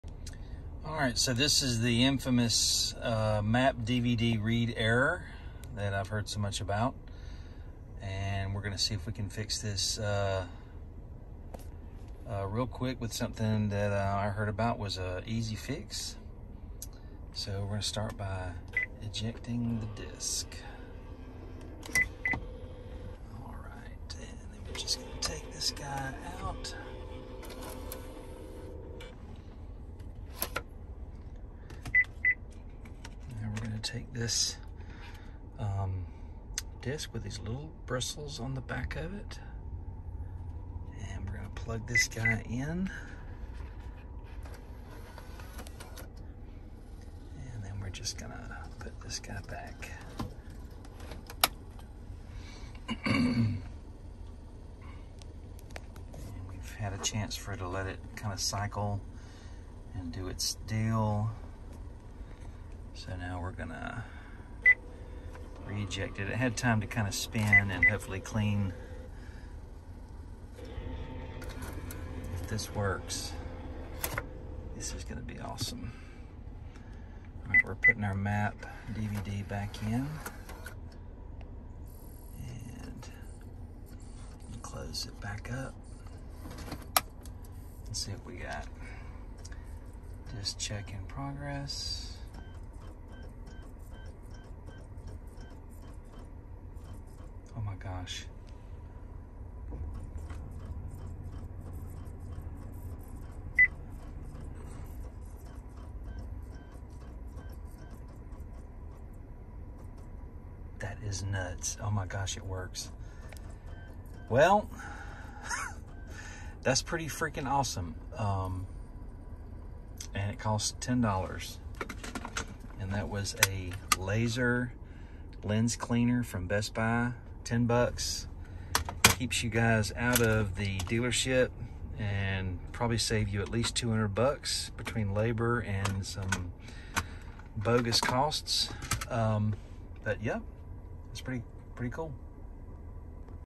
Map dvd read error fix Buick enclave
This fix for the 2011 Buick enclave map DVD read
error was easy and inexpensive fix that kept vehicle out of the shop. ￼